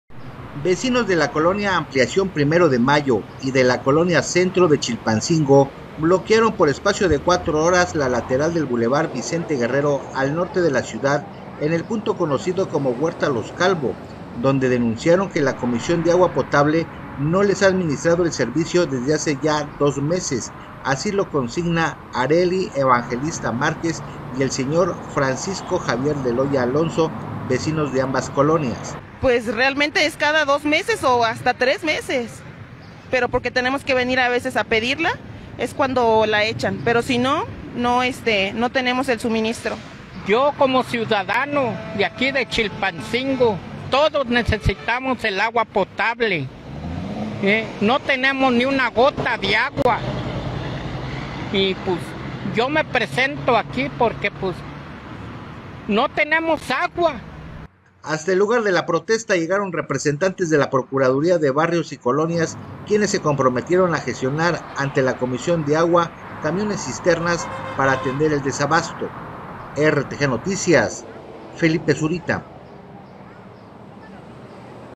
RTGnoticias | Bloquean el norte de Chilpancingo para exigir agua
Vecinos de la Colonia ampliación primero de Mayo y de la Colonia centro de Chilpancingo bloquearon Por espacio de 4 horas la lateral del bulevar Vicente Guerrero al norte de la ciudad en el punto conocido como la huerta de los calvo donde denunciaron que la comisión de agua potable no les ha administrado el servicio desde hace dos meses así lo consigna Arely evangelista Márquez y el señor Francisco Javier Deloya Alonso. Vecinos de ambas colonias.